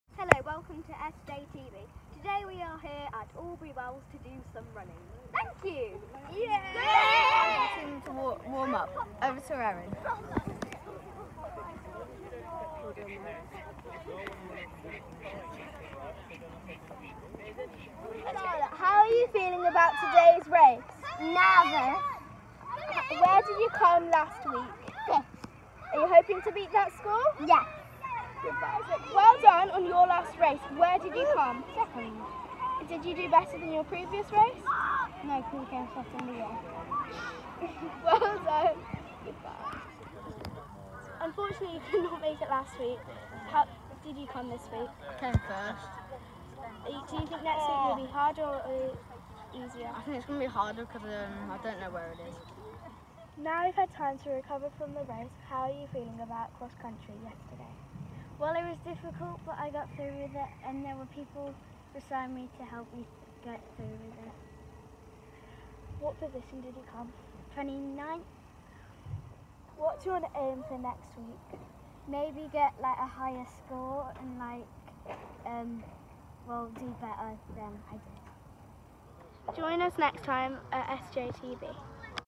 X country 2021
KS2 took part in 3 races during September, here is a vlog from a few of the children.